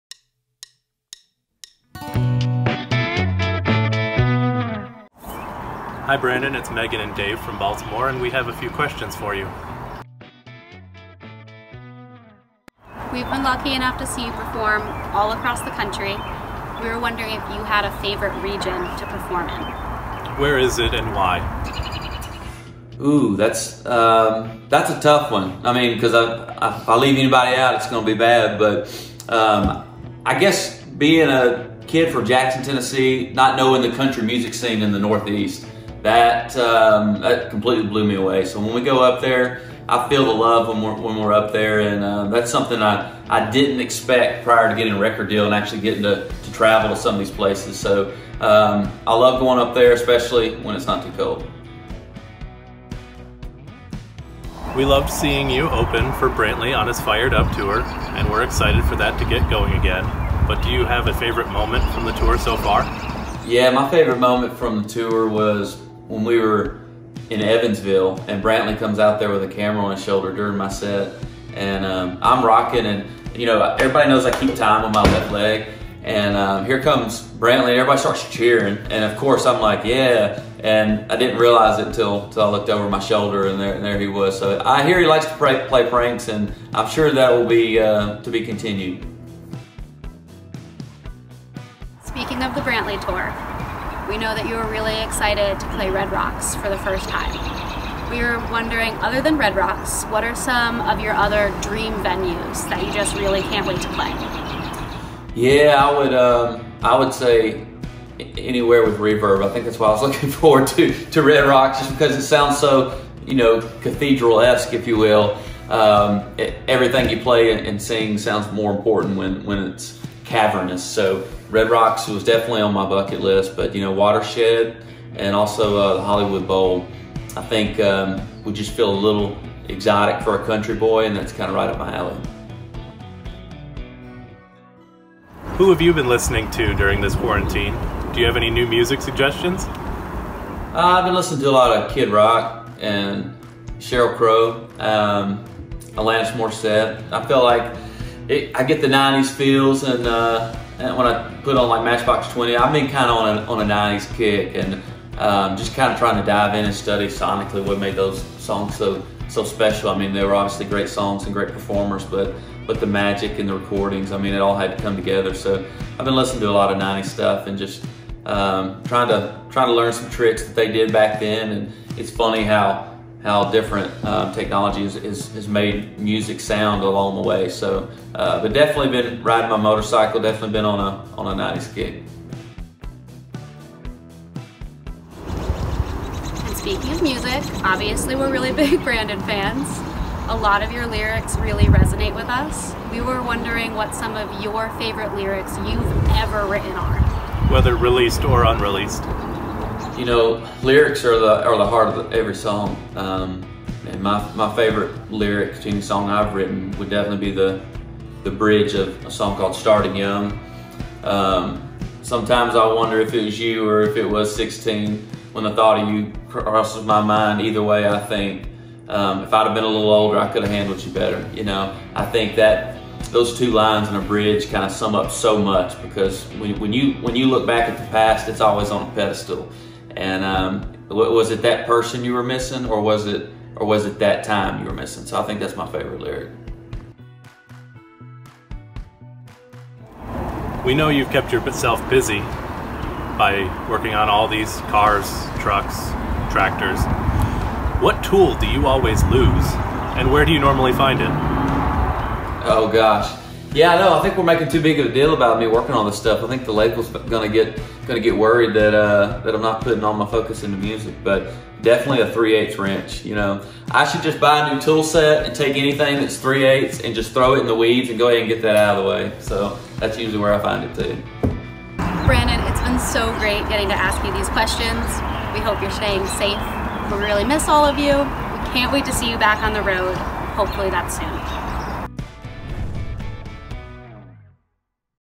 5Questions | Megan Tobin and Dave Kemner
Talked about music I've been listening to these days and some of my favorite lyrics.. Thanks for the questions Megan and Dave